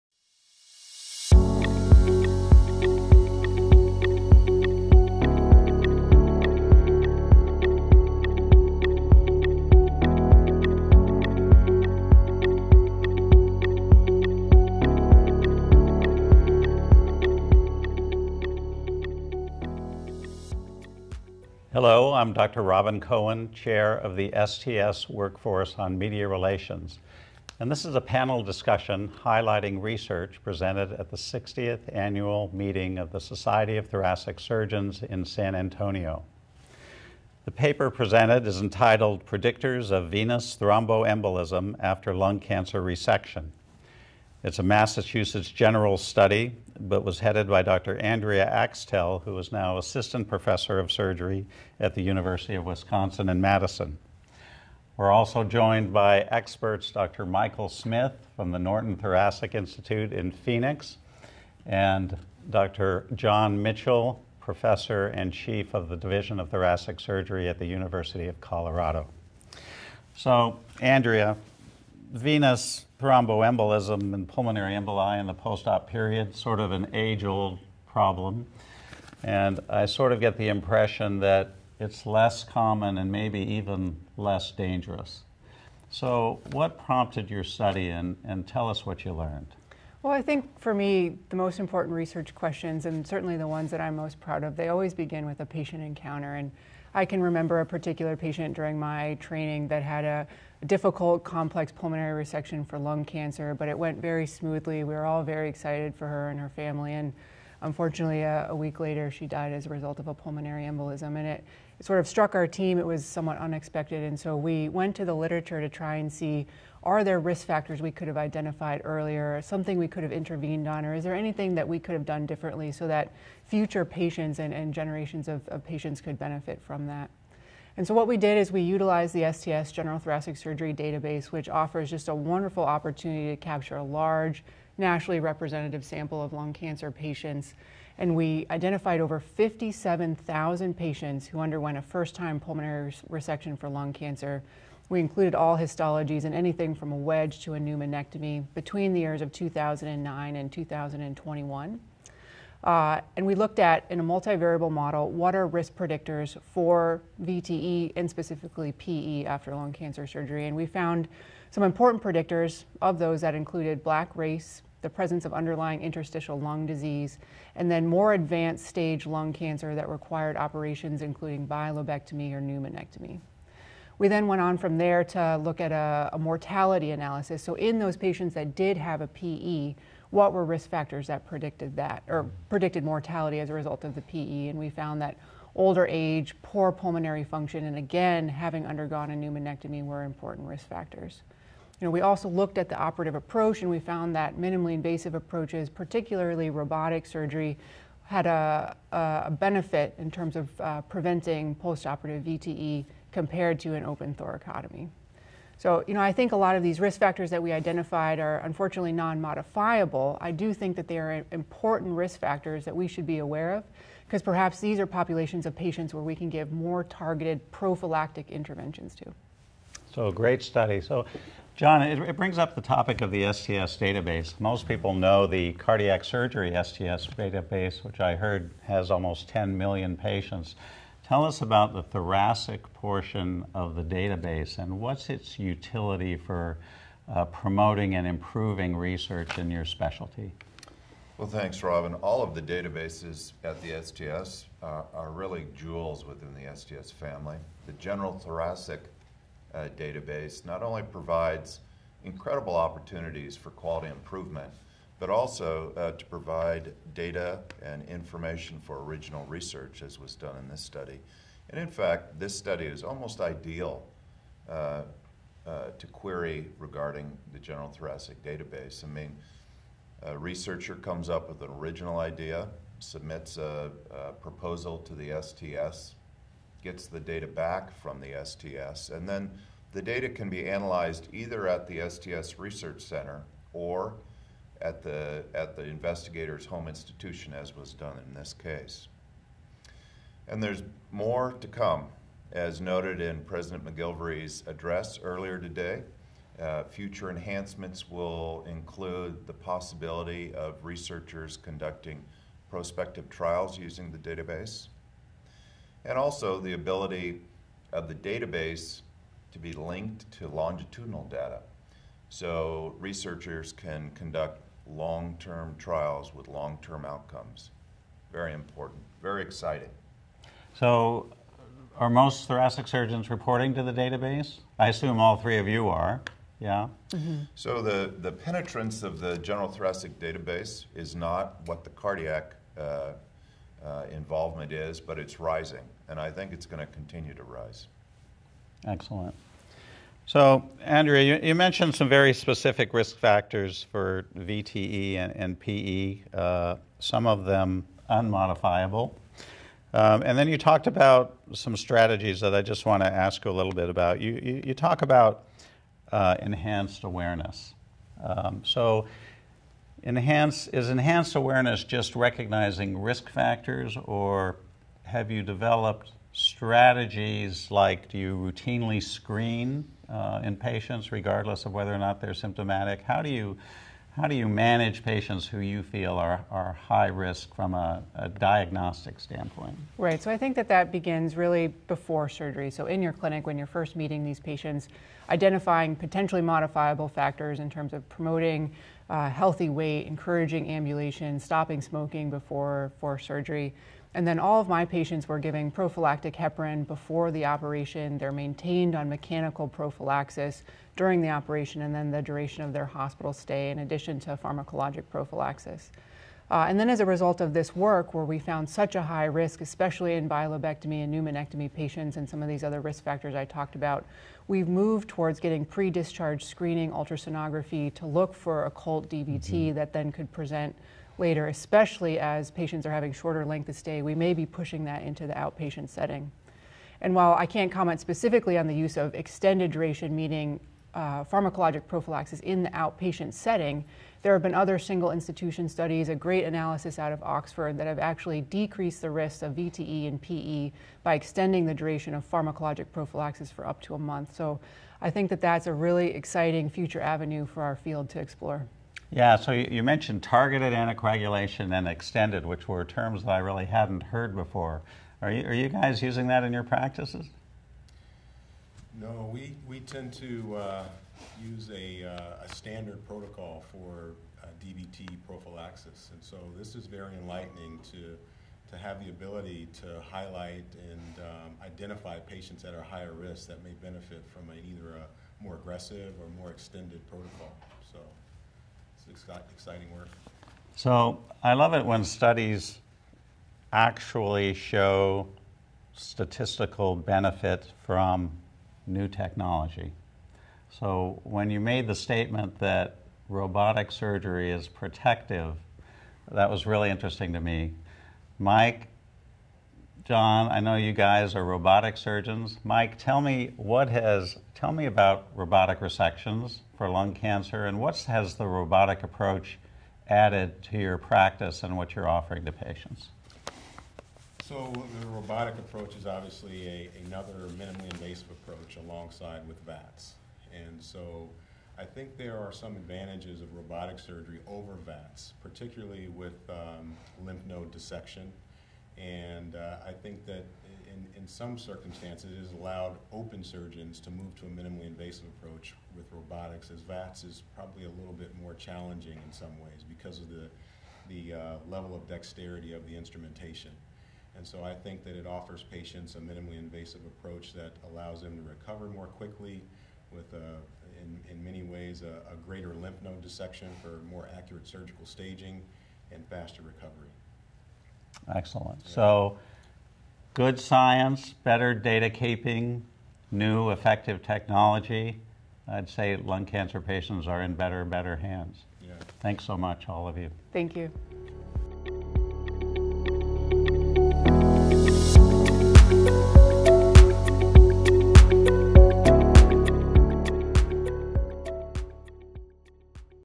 STS Panel Discussion: Predictors of Venous Thromboembolism after Pulmonary Resection for Lung Cancer
In this panel discussion, Andrea L. Axtell, MD, an assistant professor of surgery at the University of Wisconsin School of Medicine and Public Health, offers an insider look at a new study that found patients with postoperative PE had increased 30-day mortality, reintubation, and readmission rates, which was presented at the 2024 STS Annual Meeting. The discussion is moderated by Robbin Cohen, MD, Cedars-Sinai at Huntington Hospital, and includes Michael Smith, MD, Norton Thoracic Institute, St. Joseph’s Hospital & Medical Center, and John Mitchell, MD, University of Colorado Health.